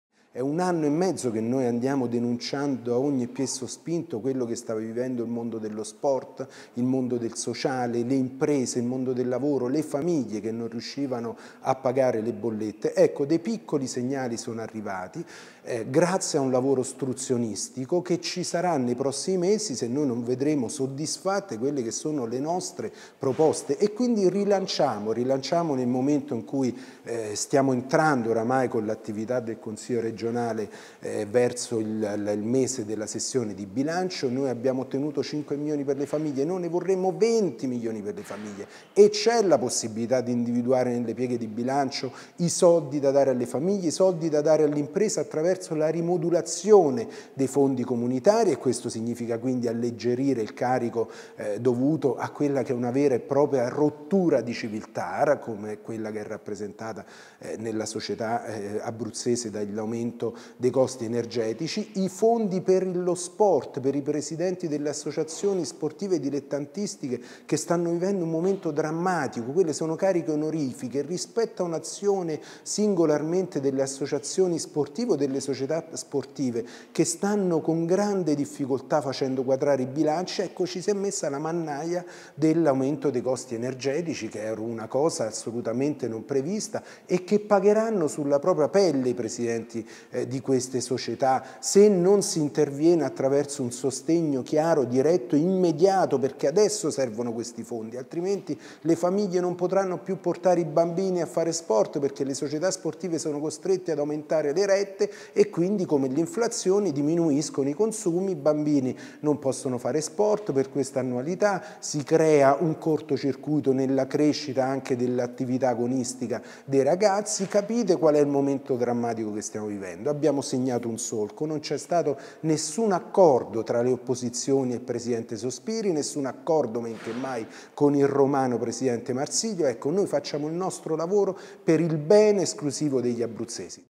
Pietrucci (PD) su Consiglio: "Nessun accordo con la maggioranza. Momento drammatico"
I Consiglieri regionali di centrosinistra, in conferenza stampa questa mattina a Palazzo dell'Emiciclo, hanno fatto il punto sui provvedimenti approvati nell'ultimo Consiglio regionale e per voce del consigliere Pierpaolo Pietrucci, hanno sottolineato il dissenso verso alcune scelte e atteggiamenti dell'Esecutivo regionale.